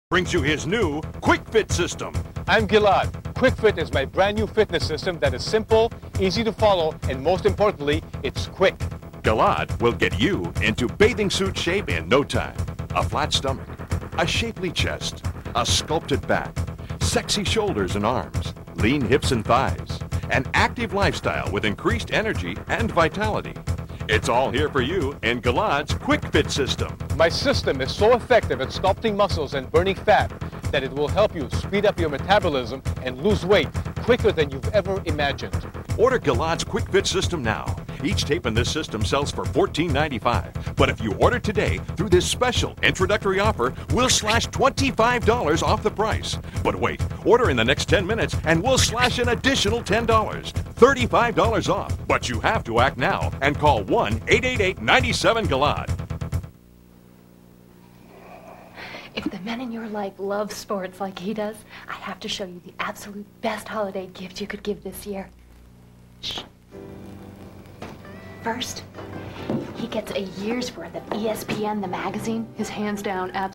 Digital Cable Guide Menu - November 13 2002 - The Health Network
Time Warner Digital Cable Guide Menu - November 13 2002 - The Health Network